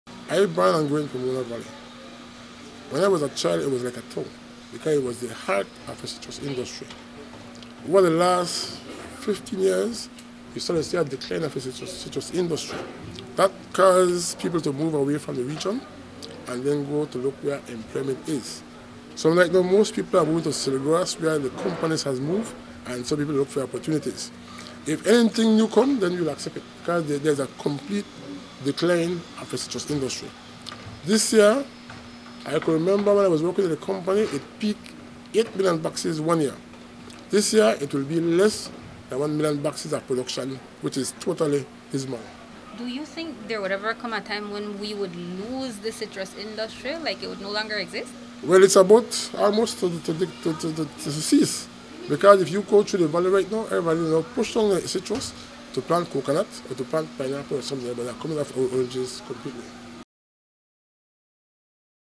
Citrus Industry Hanging by a Thin Tread, Welcome Bamboo Industry
The once thriving citrus industry in Belize has been declining over the years and is now hanging by a thin thread.  There has been a significant loss of jobs and some farmers are diversifying their investment to other crops. Last week, Prime Minister John Briceño met with stakeholders who are proposing a bamboo industry in the south. Today, Stann Creek West Area Representative Rodwell Ferguson threw his support behind a proposed bamboo industry in southern Belize. Minister Ferguson says that the citrus industry is almost deceased.